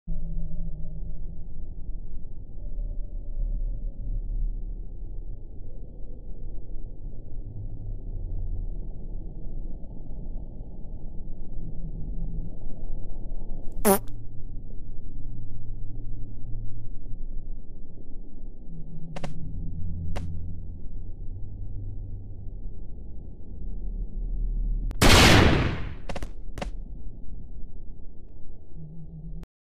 Melon is so rich!!
Melon was stealling everyone's money! But Apple put a stop to his misdeeds.